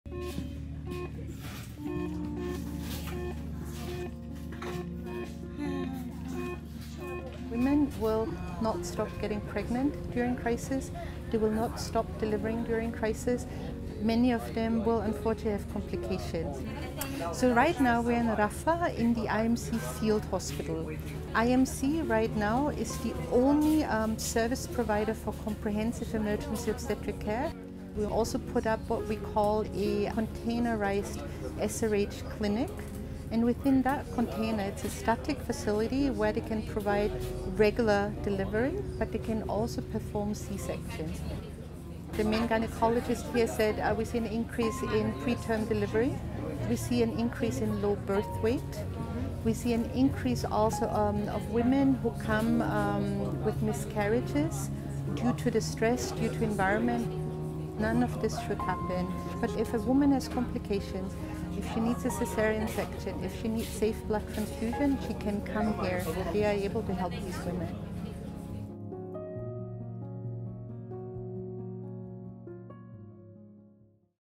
Helping women give birth safely in Gaza amid Rafah military assault
Rafah is under attack and the already desperate condition for thousands of pregnant women is getting worse every day.

See how UNFPA—the United Nations sexual and reproductive health agency—is taking action to make motherhood safer at the International Medical Corps (IMC) field hospital in Gaza as the military assault on Rafah leads to more trauma and death